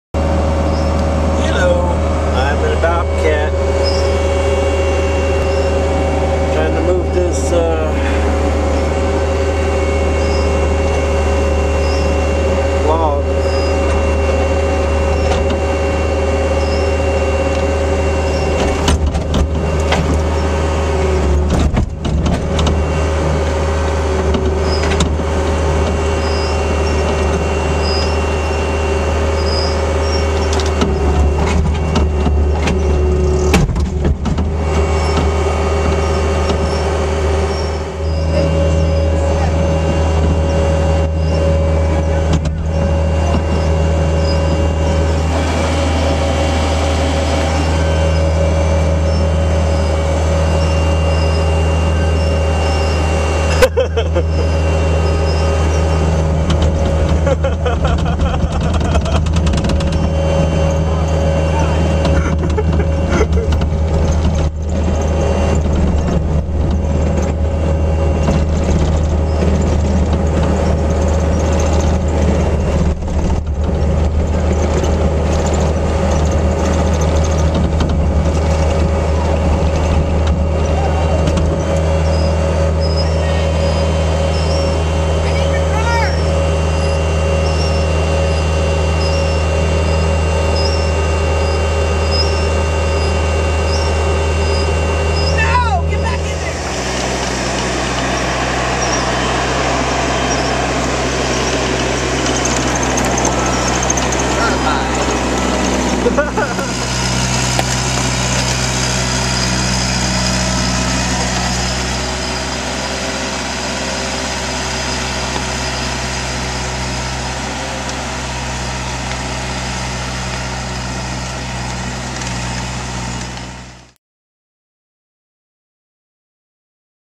Bobcat - The 40 dollar an hour toy!
A video of us moving stuff with a bobcat... including Greg in the bucket!